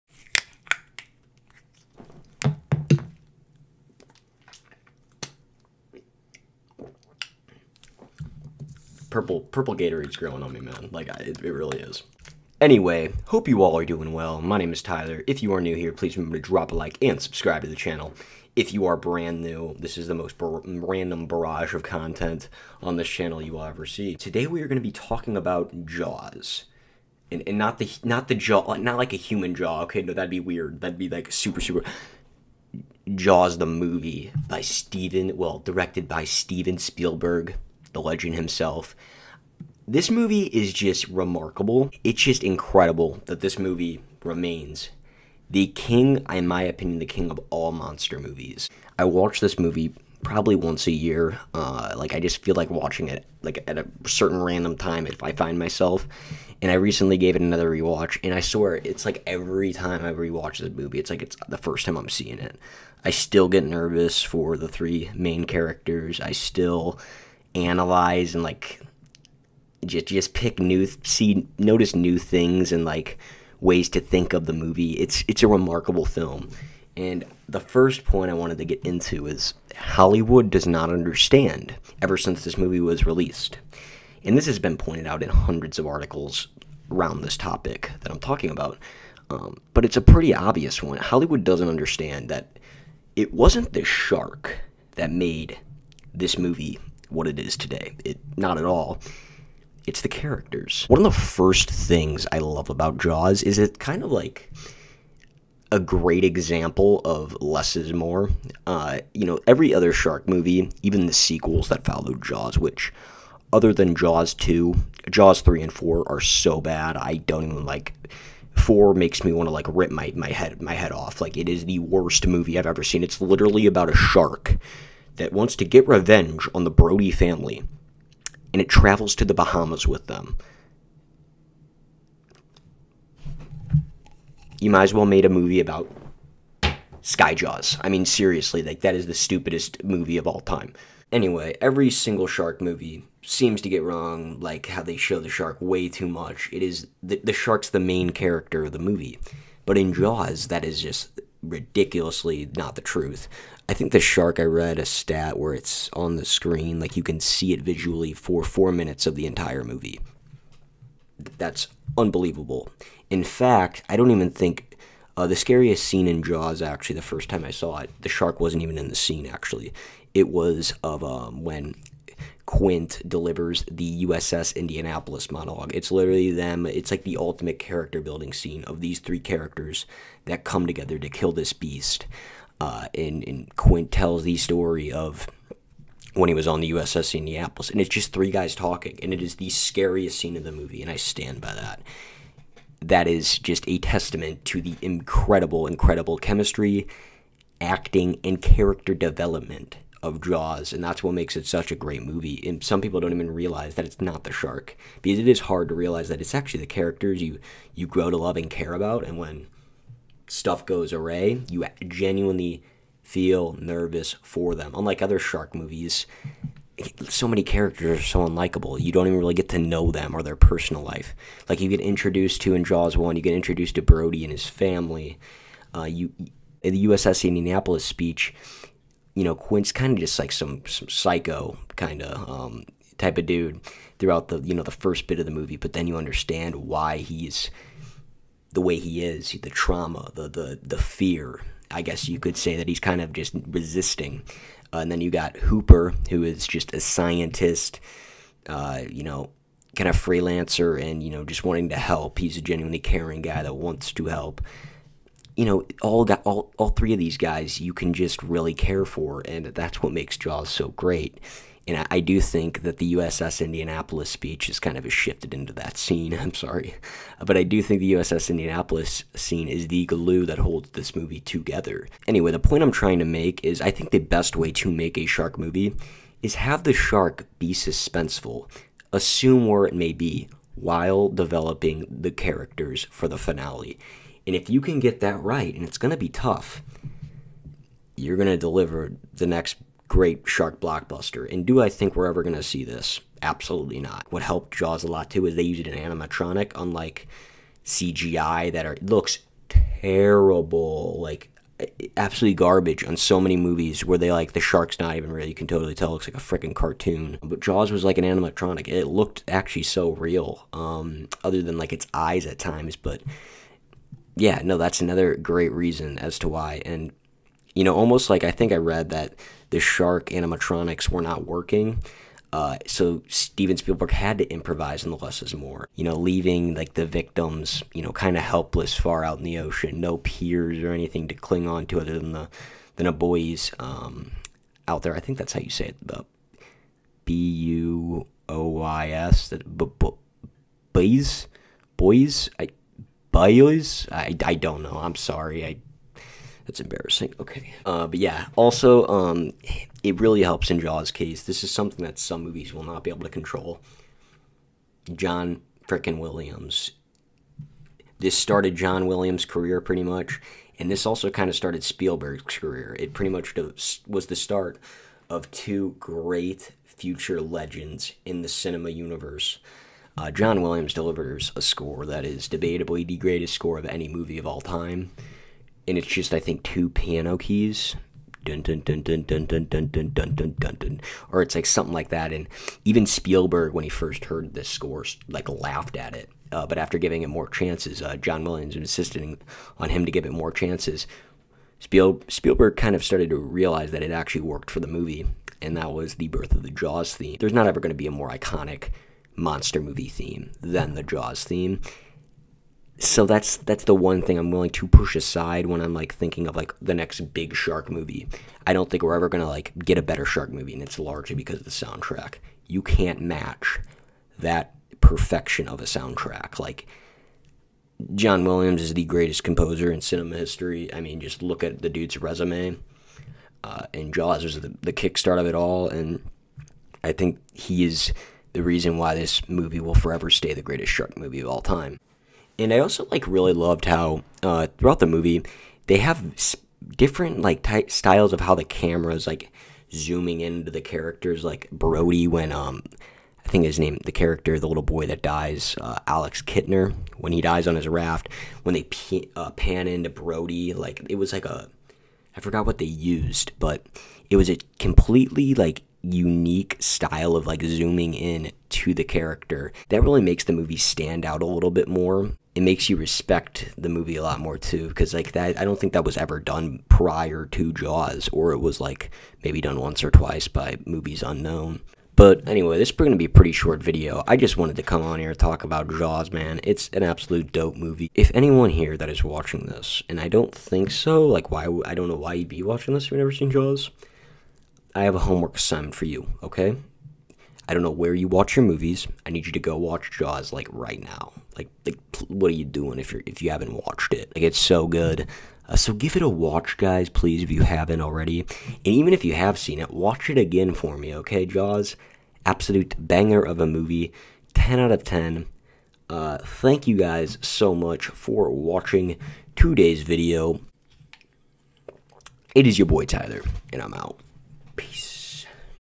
What Makes Jaws the Greatest Shark Movie of All Time!
Okay here we go! Today we talk about one of the few movies to be considered as a timeless classic & why no other shark movie has even come close to capturing what the first Jaws film was able to do. Sit back and lets get it!